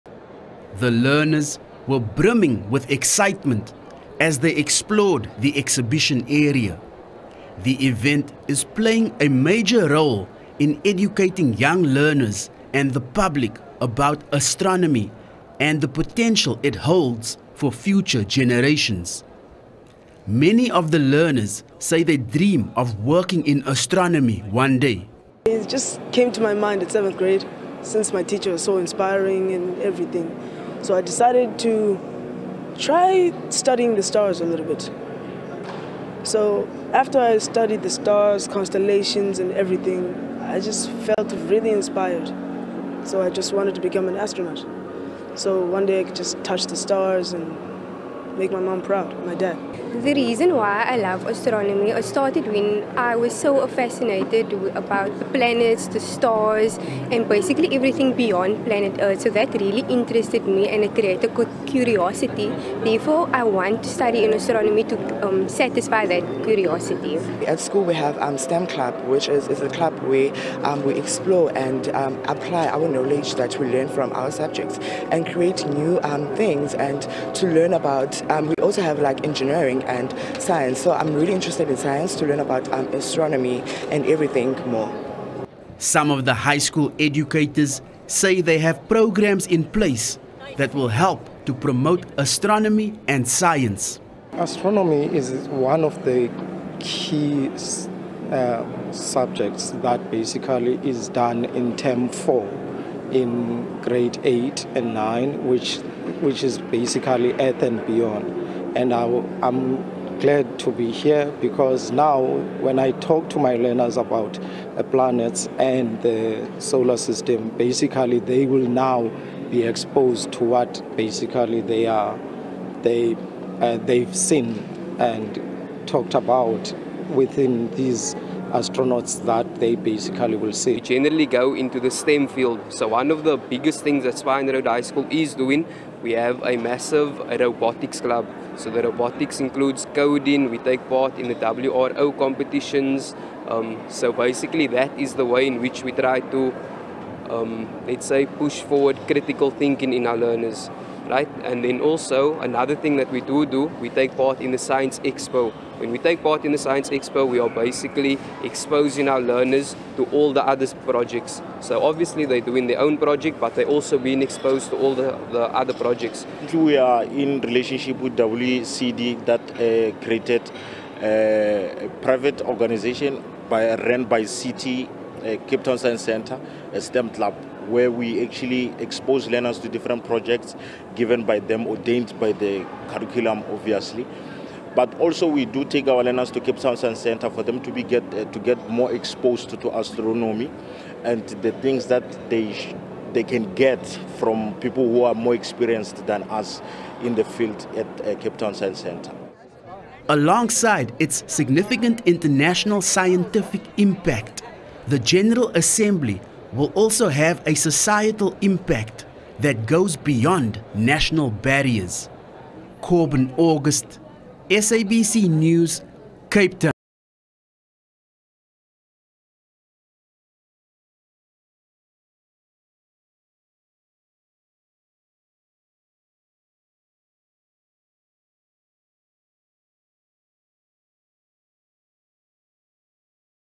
Cape Town learners navigating the stars
Learners from various schools across Cape Town had the opportunity to interact and learn about  various astronomy and science projects, when they visited the International Astronomical Union's 32nd General Assembly, currently under way in Cape Town.

The gathering is being held for the first time in its 105-year history on the African continent .

Over 2000 astronomers from more than 80 countries will participate at the gathering.